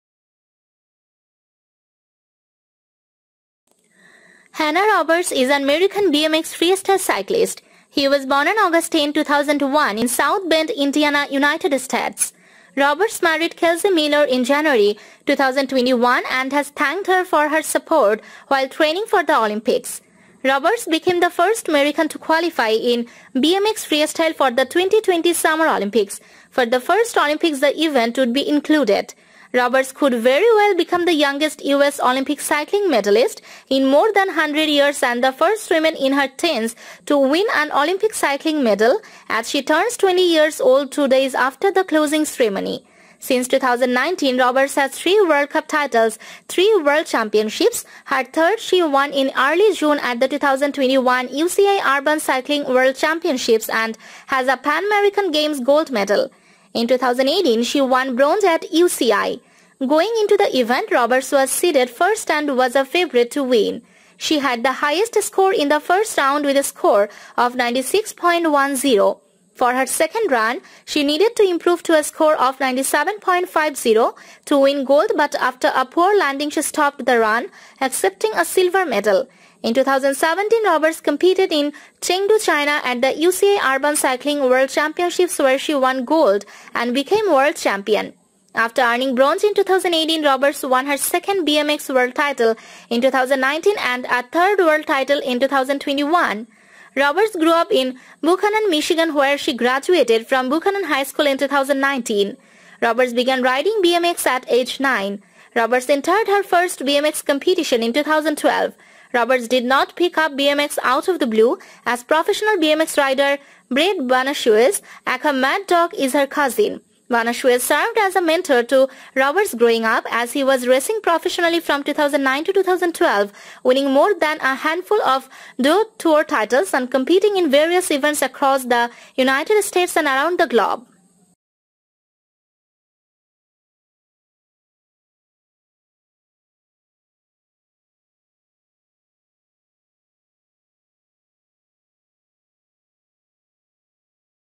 Hannah Roberts || 10 Things You Didn't Know About Hannah Roberts
Hannah Roberts is an American BMX freestyle cyclist.
He was Born on August 10, 2001 in South Bend, Indiana, United States.
Roberts married Kelsey Miller in January 2021 and has thanked her for her support while training for the Olympics.
Roberts became the first American to qualify in BMX freestyle for the 2020 Summer Olympics, for the first Olympics the event would be included..